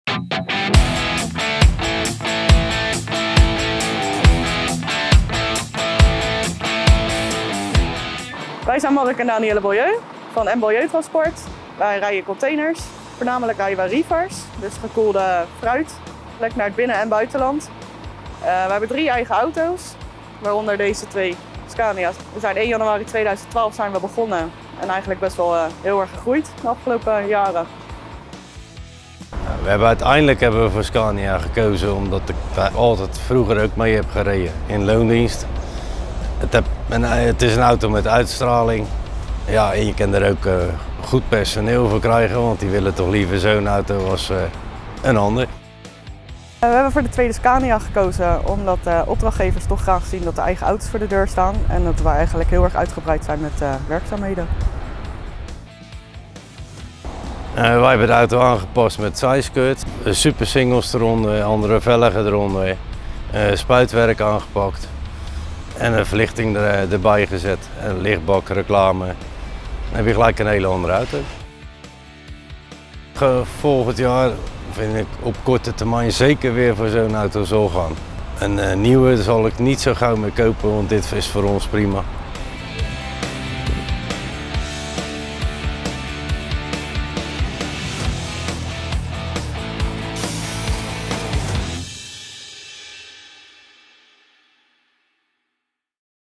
Scania Talent Used Truck: M Baljeu Transport heeft er 2 rondrijden
M Baljeu Transport heeft 2 Scania Talent jong gebruikte auto's rondrijden. Na wat aanpassingen rijden ze nu op de binnen en buitenlandse wegen. Meer weten over de Scania Talent? Kijk dan op